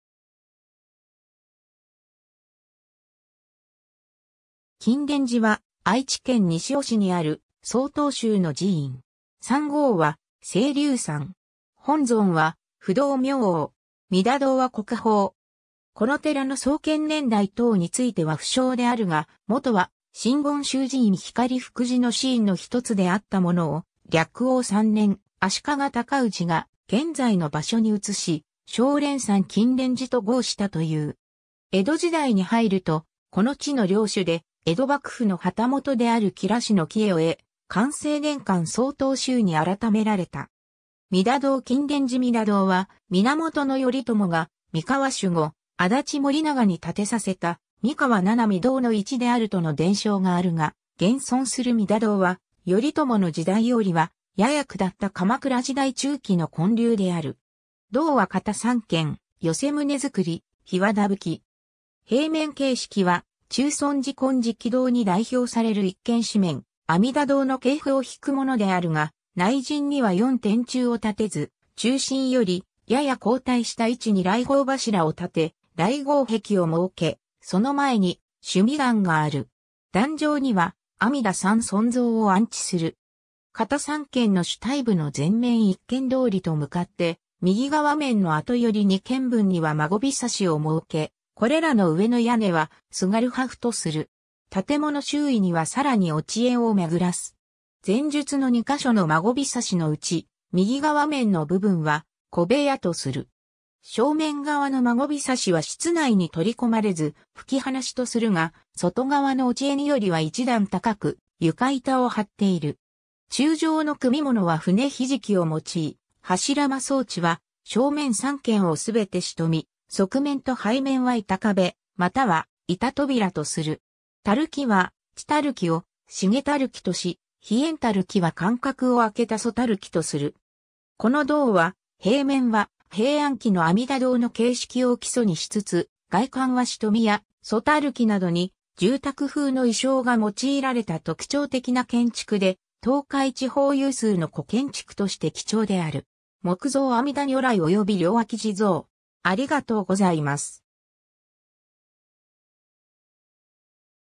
金蓮寺 (西尾市)
日本の国宝_(建造物)
金蓮寺（こんれんじ）は、愛知県西尾市にある曹洞宗の寺院。
山号は青龍山。
本尊は不動明王。
弥陀堂は国宝。
 この寺の創建年代等については不詳であるが、もとは真言宗寺院光福寺の子院のひとつであったものを、暦応3年（1340年）足利尊氏が現在の場所に移し、青蓮山金蓮寺と号したという。
江戸時代に入ると、この地の領主で江戸幕府の旗本である吉良氏の帰依を得、寛政年間（1789年 - 1801年）曹洞宗に改められた。
 弥陀堂 金蓮寺弥陀堂は源頼朝が三河守護安達盛長に建てさせた「三河七御堂」の一であるとの伝承があるが、現存する弥陀堂は頼朝の時代よりはやや下った鎌倉時代中期の建立である。
堂は方三間（桁行3間、梁間3間）、寄棟造、檜皮葺。
平面形式は中尊寺金色堂に代表される一間四面阿弥陀堂の系譜を引くものであるが、内陣には四天柱を立てず、中心よりやや後退した位置に来迎柱を立て、来迎壁を設け、その前に須弥壇がある。
壇上には阿弥陀三尊像を安置する。
方三間の主体部の前面一間通りと向かって右側面の後寄り2間分には孫庇を設け、これらの上の屋根は縋破風（すがるはふ）とする。
建物周囲にはさらに落縁をめぐらす。
前述の2か所の孫庇のうち、右側面の部分は小部屋とする。
正面側の孫庇は室内に取り込まれず、吹き放しとするが、外側の落縁よりは一段高く床板を張っている。
柱上の組物は舟肘木を用い、柱間装置は正面3間をすべて蔀（しとみ）、側面と背面は板壁または板扉とする。
垂木は地垂木を繁垂木（しげだるき）とし、飛檐垂木（ひえんだるき）は間隔を空けた疎垂木（まばらだるき）とする。
この堂は、平面は平安期の阿弥陀堂の形式を基礎にしつつ、外観は蔀や疎垂木などに住宅風の意匠が用いられた特徴的な建築で、東海地方有数の古建築として貴重である。
 木造阿弥陀如来及び両脇侍像